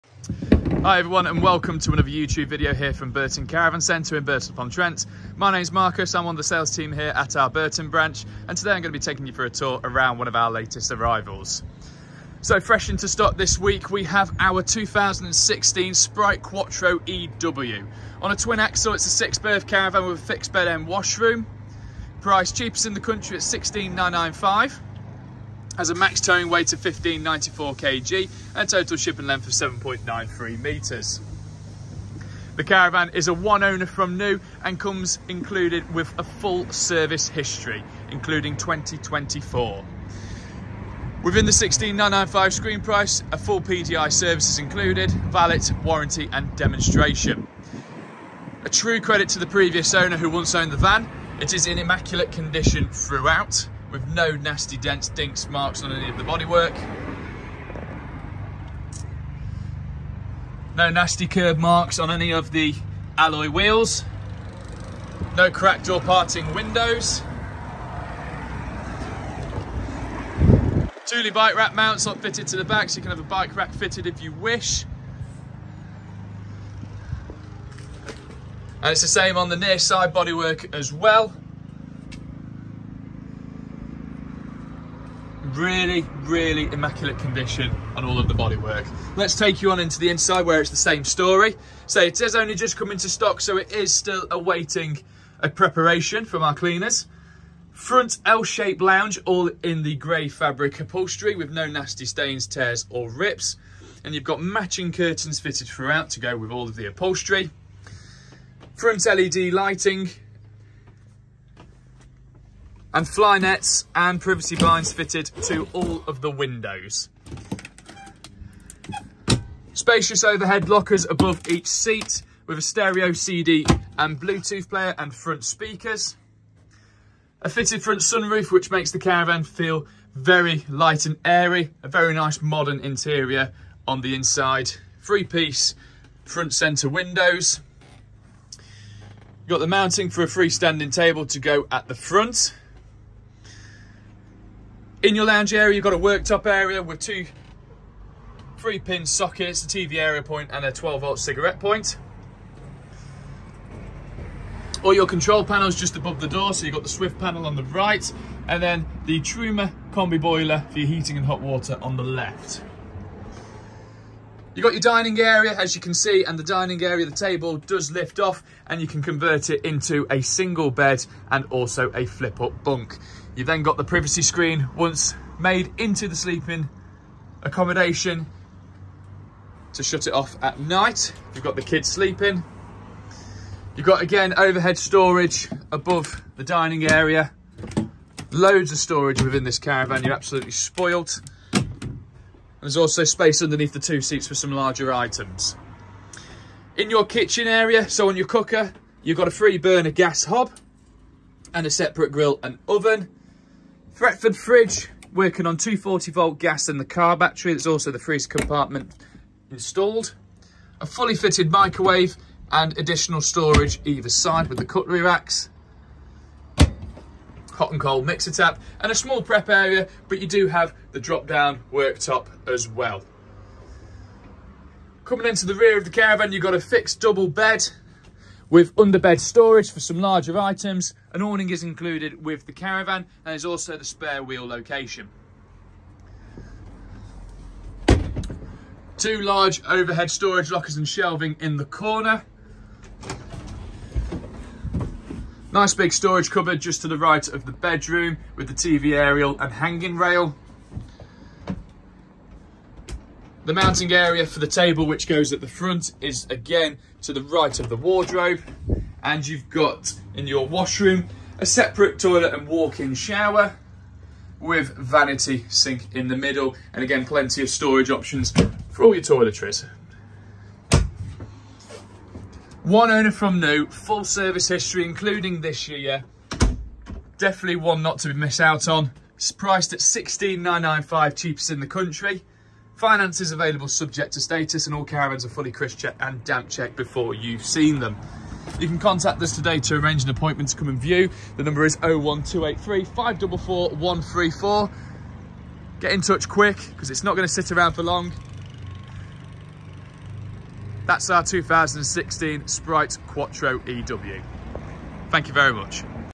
2016 Swift Sprite Quattro EW
2016 Swift Sprite Quattro EW!
6 Berth, Fixed Bed
Twin Axle!
End washroom
“L” Shaped Lounge
Max tow weight - 1583 kg
Total shipping length - 7.93 m
1 Owner from new!!
FULL SERVICE HISTORY!!
Awning Including and loads more!!

Price - £16,995! Cheapest in the country!!

Included with the caravan is a PDI Service, Valet, Warranty & Demonstration

BURTON CARAVAN CENTRE
167 BURTON ROAD
BRANSTON
BURTON UPON TRENT
DE14 3DP